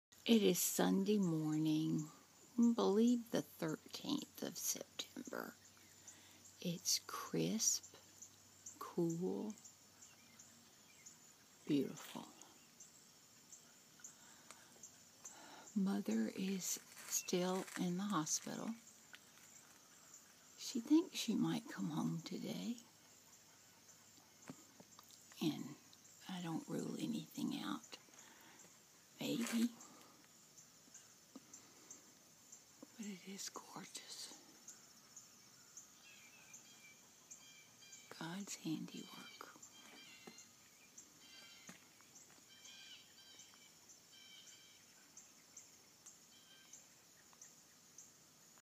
My Mother's yard on a Sunday morning Sept. 14,2014
Beautiful crisp cool morning in West Tennessee